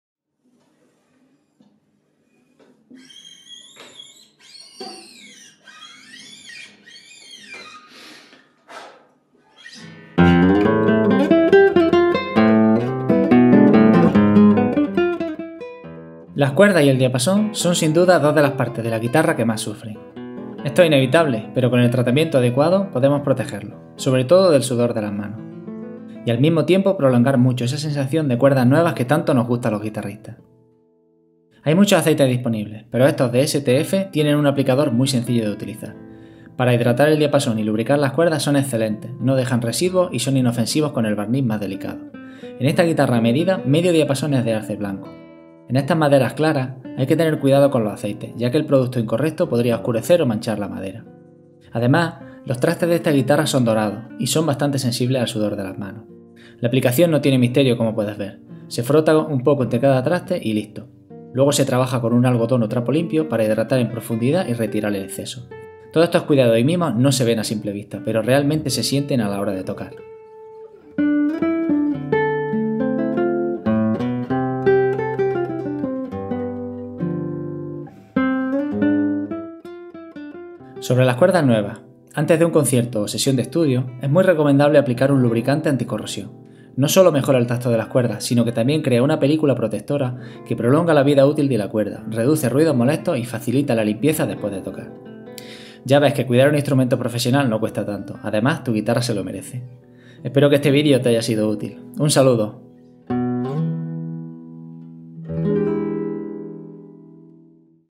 Secretos de Guitarrero #05 · Aceite para diapason y cuerdas
En este vídeo muestro el proceso de aplicación de los aceites de STF para diapasón y cuerdas.